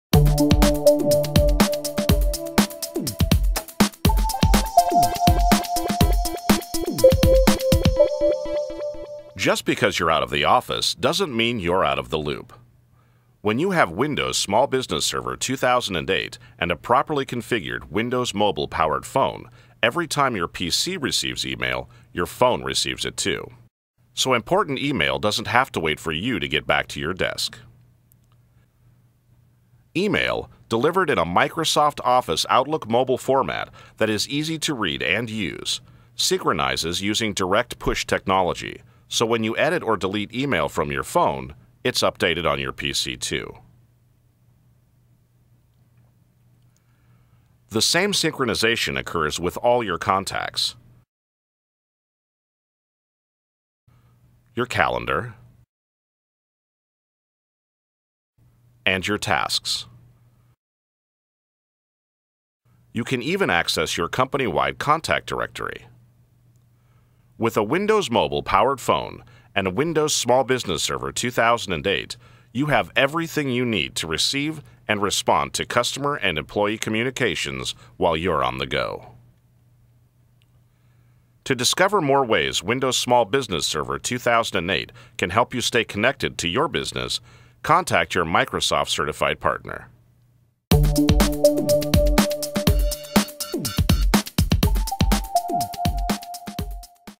Stay Connected With Email On Your Phone
Stay in the loop when you are out of the office.  This demo short details how with Windows Small Business Server 2008 and a properly configured Windows Mobile-powered phone, every time your PC receives e-mail, your phone can receive it, too.